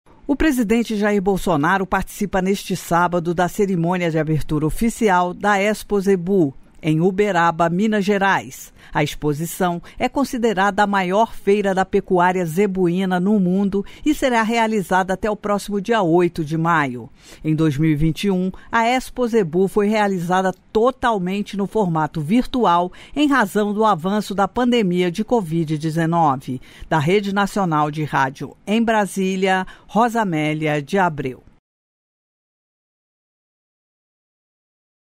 30/04/2022 - BOLSONARO PARTICIPA DA EXPOZEBU, EM UBERABA (MG)
Exposição é considerada na maior feira da pecuária zebuína no mundo e será realizada até o próximo dia 8 de maio.

O presidente Jair Bolsonaro participa neste sábado (30) Cerimônia de Abertura Oficial da  ExpoZebu”, em Uberaba Minas Gerais .

A exposição é considerada na maior feira da pecuária zebuína no mundo e será realizada até o próximo dia 8 de maio.

Em 2021, a  ExpoZebu foi realizada totalmente no formato virtual em razão do avanço da pandemia de Covid-19.

registrado em